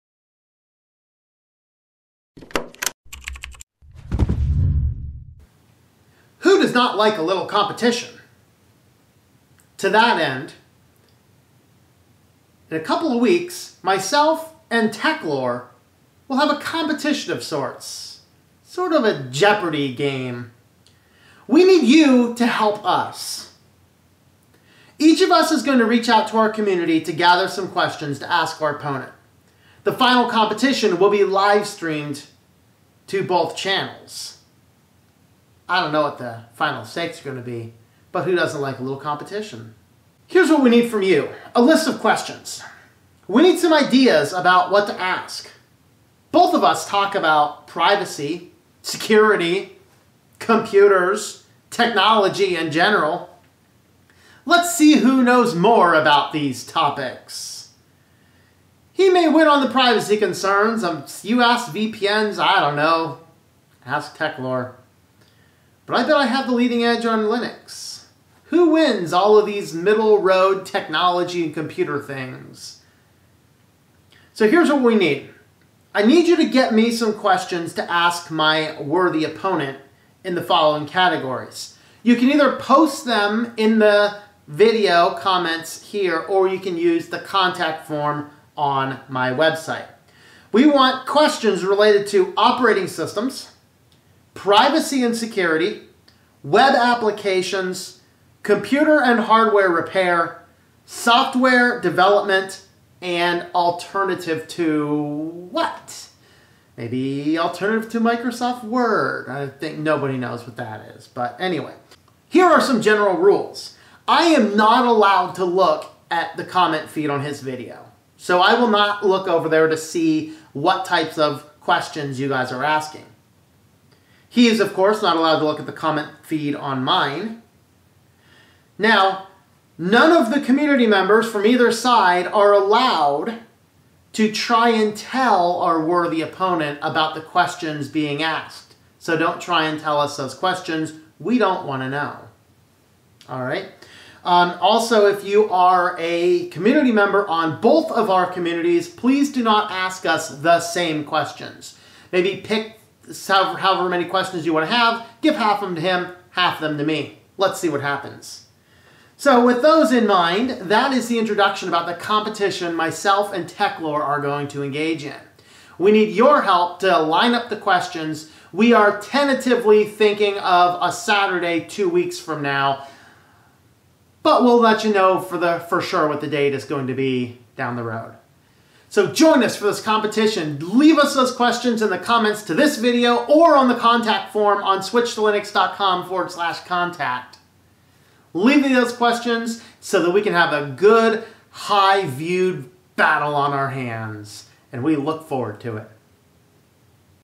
STL vs. Techlore!! I need your help!
We need your help!  Myself and Techlore will be having a trivia competition in a few weeks.  I need some questions from you on the following topics:
Operating Systems
Privacy and Security
Web Applications
Computer Hardware and repair
Software Development
Alternative To...

He cannot look in my comment feed so you can leave them here.  You can also reach out on my contact form